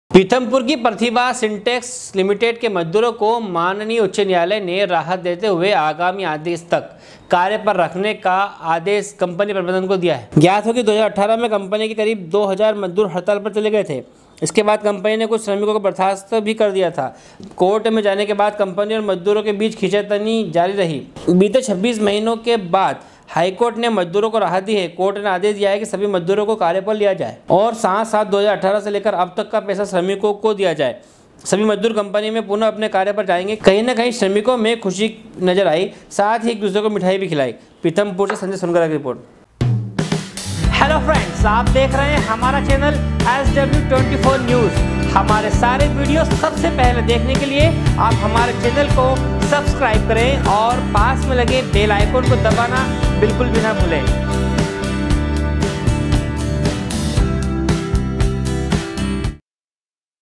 SW 24 News Madhyapradesh
पीथमपुर की प्रतिभा सिंटेक्स लिमिटेड के मजदूरों को माननीय उच्य न्यायलय ने राहत देते हुवे आगामी आदेश तक कार्य पर रखने का आदेश कंपनी प्रबंधन को दिया है। पीथमपुर से संजय सोनगरा की रिपोर्ट